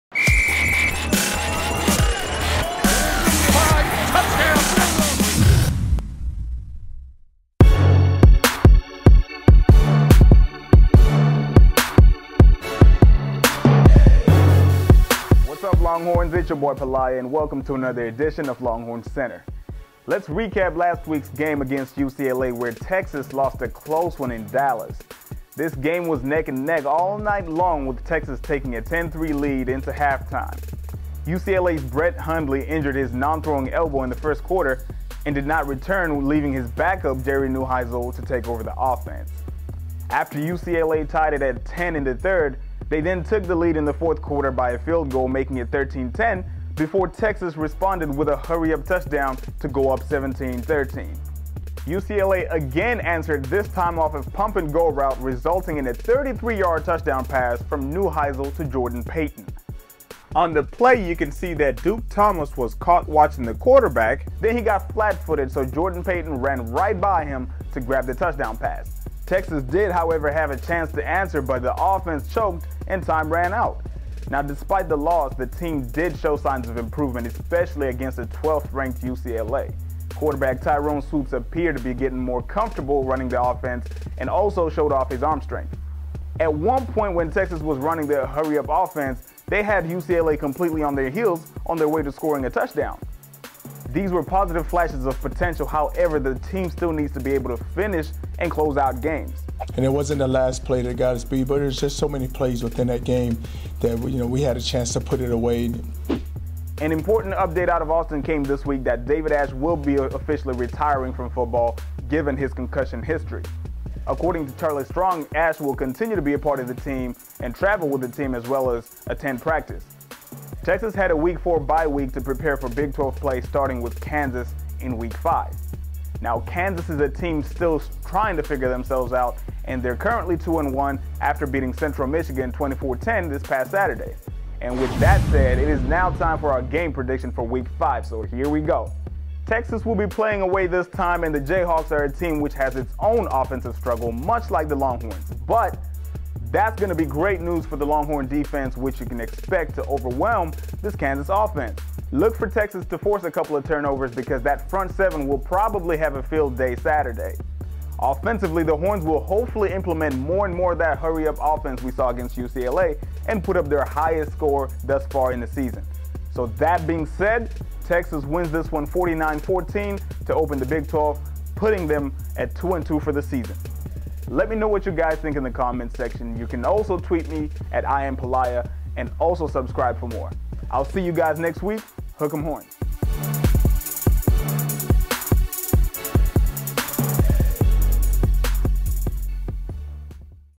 2014 Texas vs. Kansas (UCLA Recap)
Coming off a Week 4 Bye, the Longhorns will be facing Kansas Saturday in Lawrence. Texas will need to win this game to get off to a good start in conference play. Suspended players are not expected to return this week.